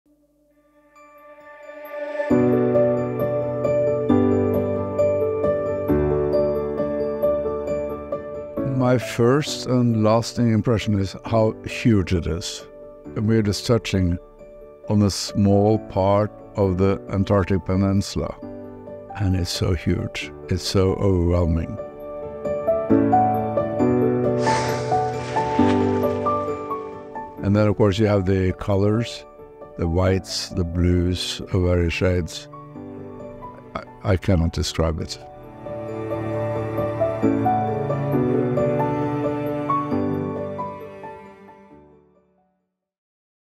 Be Curious: Viking Chairman Torstein Hagen discusses his trip to Antarctica (Trailer)
Stoke your curiosity about the icy continent of Antarctica during this exclusive conversation between Viking Chairman Torstein Hagen and award-winning British photographer Alastair Miller. Learn more about the magnificent landscapes and majestic wildlife Tor encountered during his recent expedition to Antarctica on board the Viking Polaris. Follow along as our chairman shares the intriguing story of how his lifelong passion for travel and interest in far-flung destinations began during his childhood, when he sent a letter to Ushuaia from his home in Norway.​